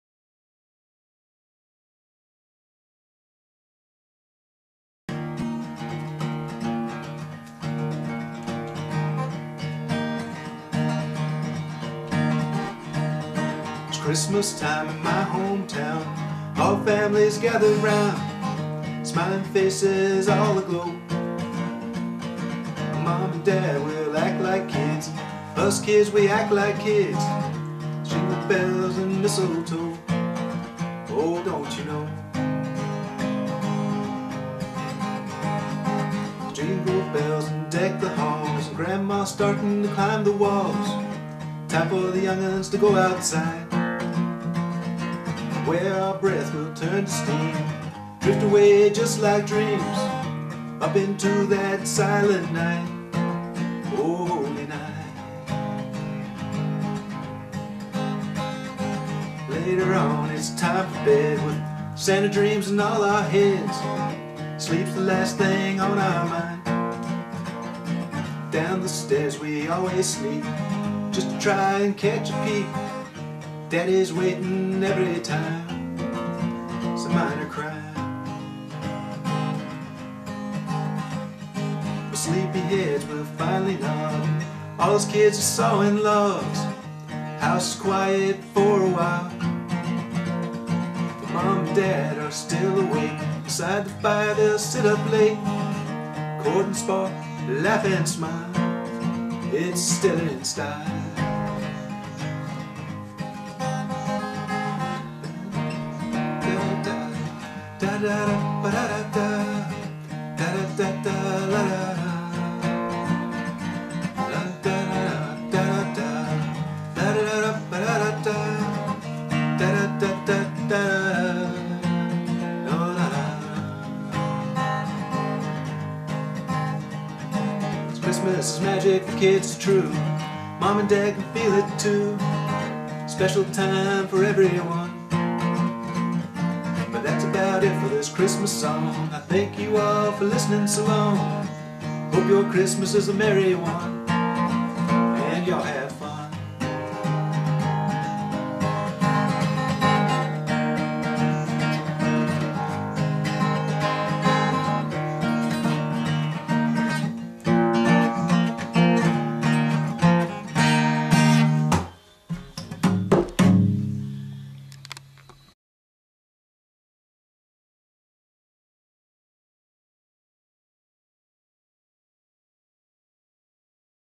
A Christmas Card
I wrote this ditty last year, and recorded it for my family.  Thought I'd "regift" by doing a live version for 2011.  Happy Christmas to all...